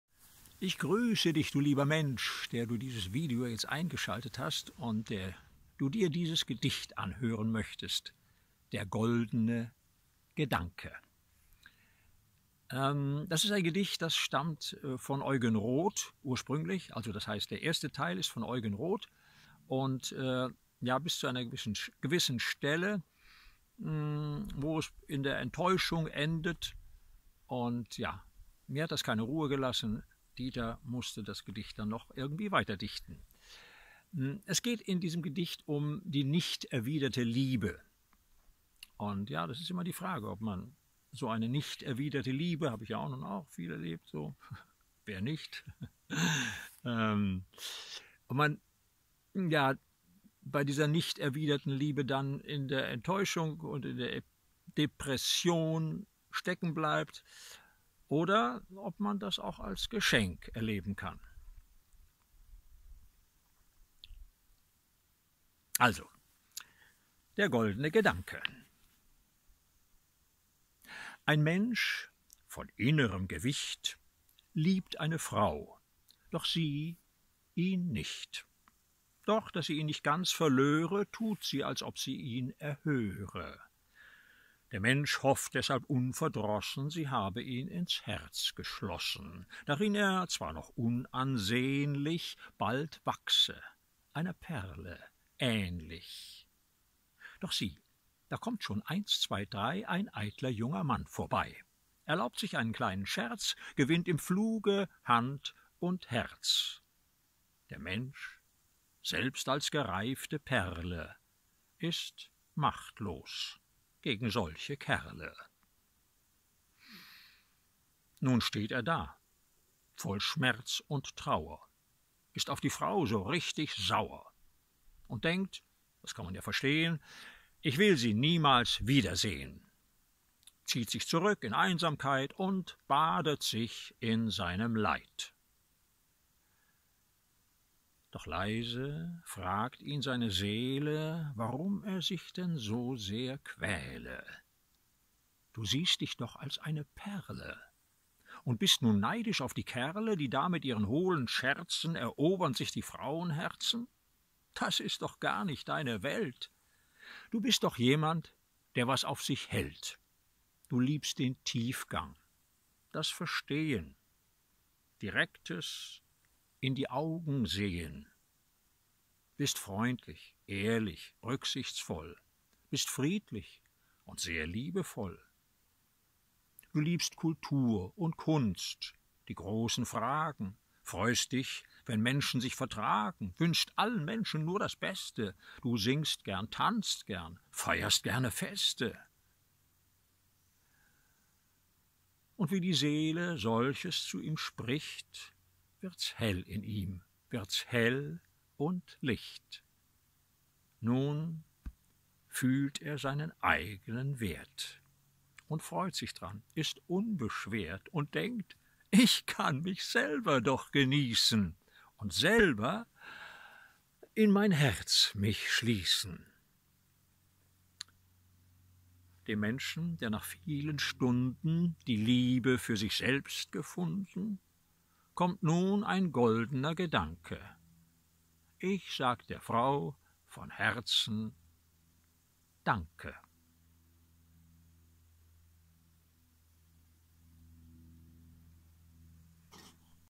"Ein Mensch von innerem Gewicht" oder "Der goldene Gedanke" (Eugen Roth und Dieter Strobel)
Ein Gedicht von Eugen Roth (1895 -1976) weitergedichtet von Dieter Strobel.